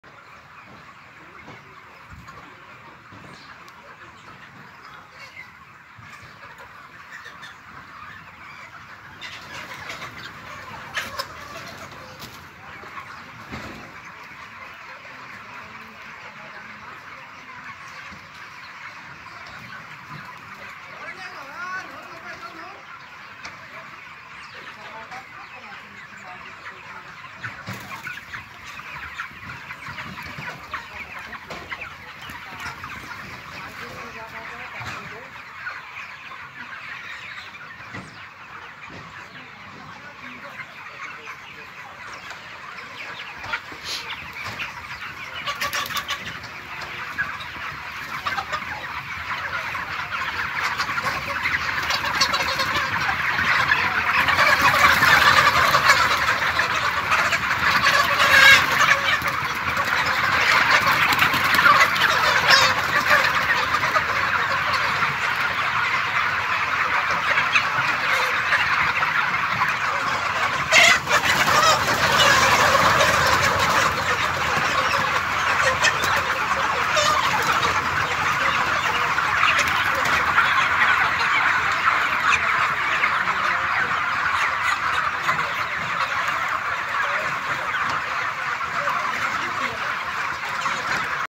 Aves morrendo por falta de eletricidade em granja de Tocantinópolis
Energia elétrica foi desligada para manutenção da rede sem que a proprietária do aviário fosse avisada da interrupção